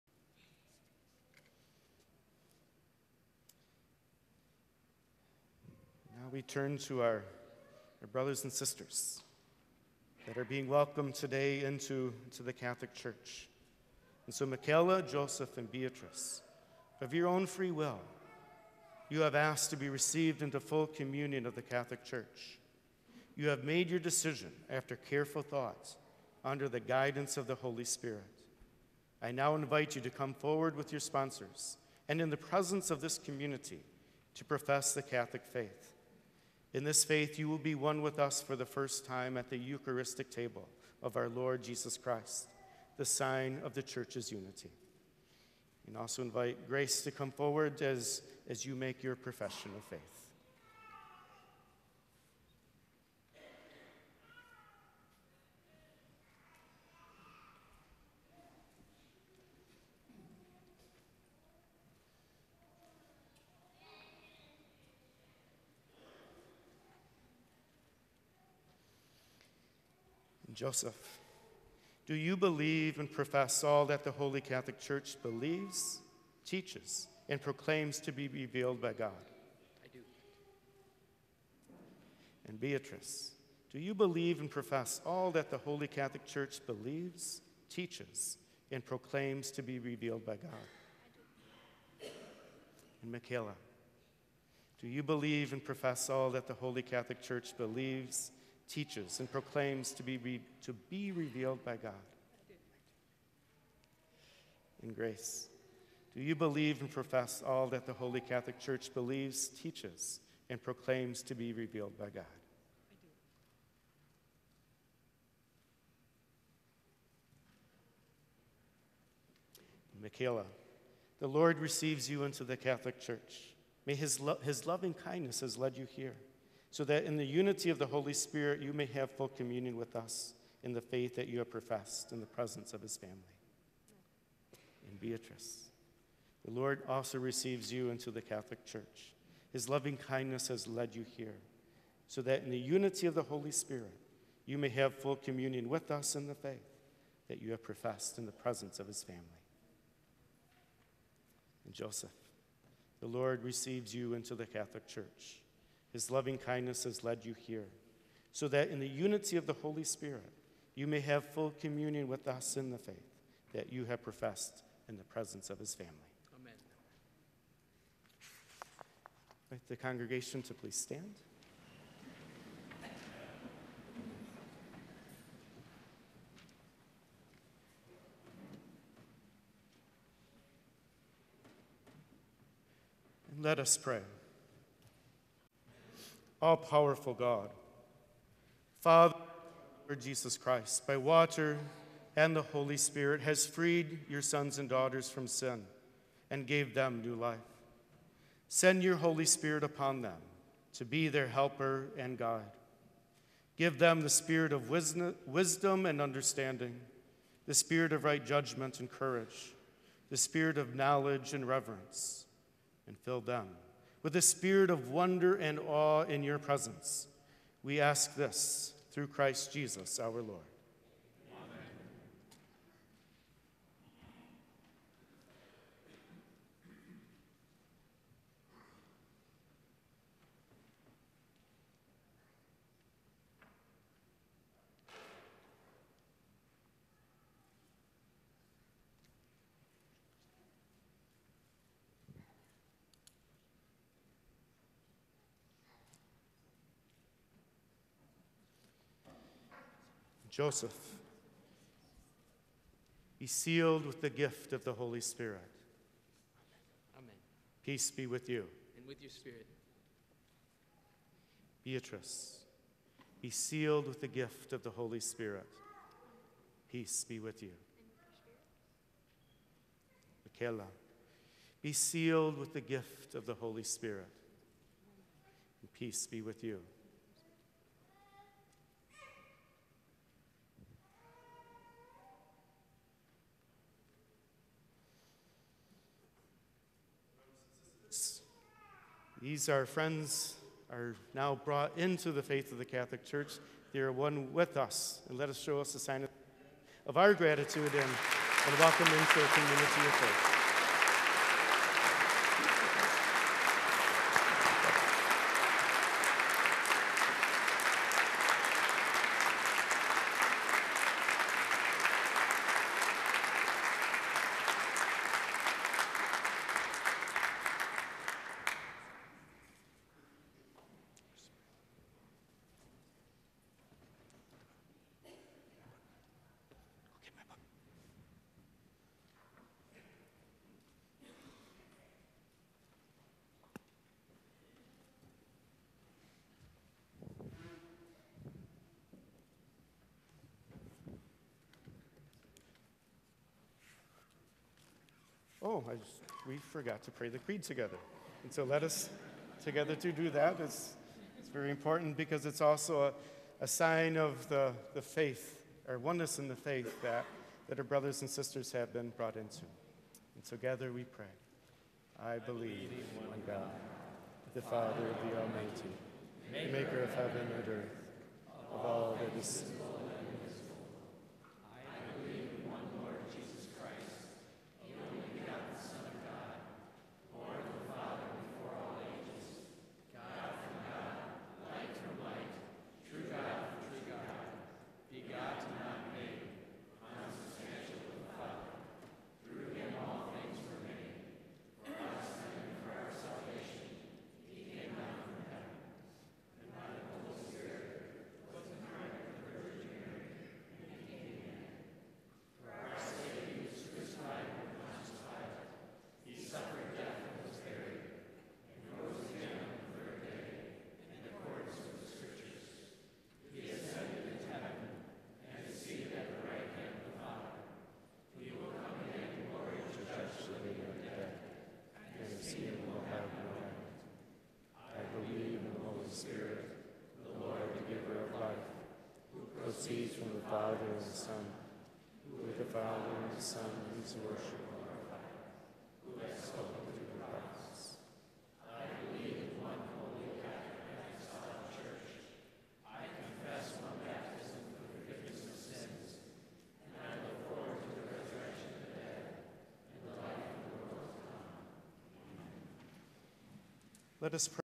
Rite of Full Reception into the Roman Catholic Church, November 26, 2017
We welcomed four people into the Catholic Church.  Let us continue to pray for them as they continue their faith journey with us here at St. Francis de Sales.